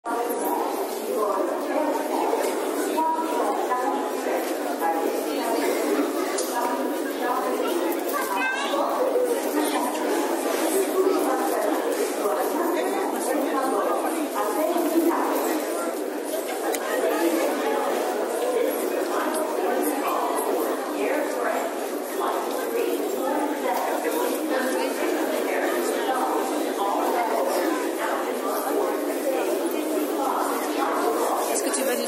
Raël à l'aéroport de Montréal
Arriving from Paris, Rael in Montreal's airport. The young girls at 0:15 are going to satisfy him later in the same night.